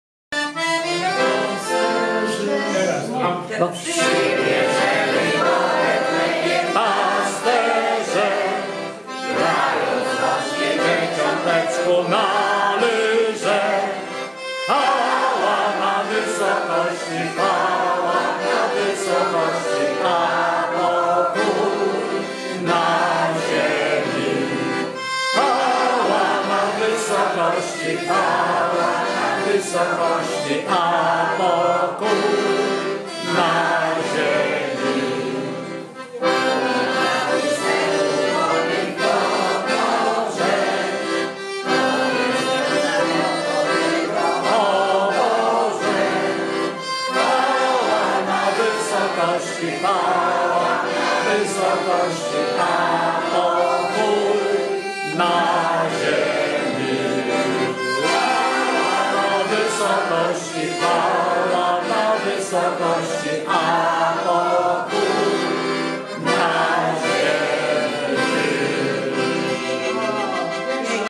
Wigilia w Kole Nadzieja w Sierpcu
W środę, 18 grudnia 2019 r., odbyło się spotkanie opłatkowe w Miejsko-Gminnym Kole PSD Nadzieja w Sierpcu.